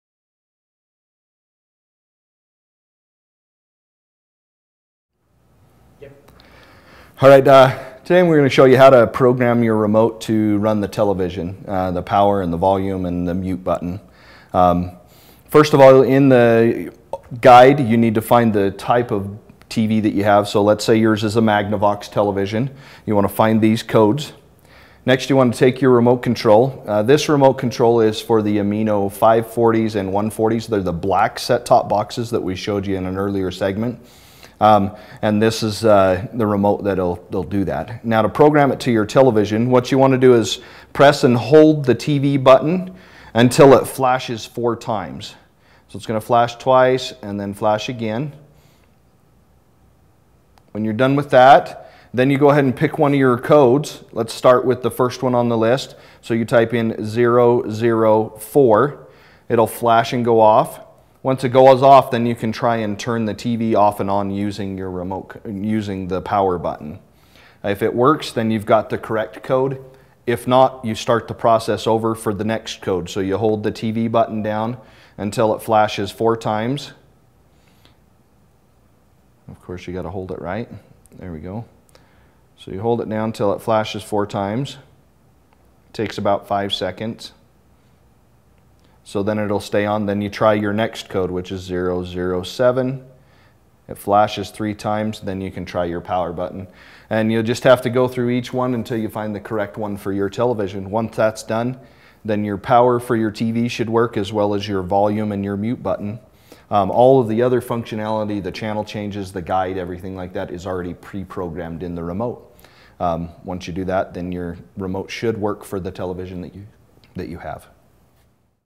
Amino 530 Remote (Program and Function)
How to program your Amino 530 remote to your T.V. for the volume and power keys. Also the function of the buttons in the guide menu with Manti Tele Communications service.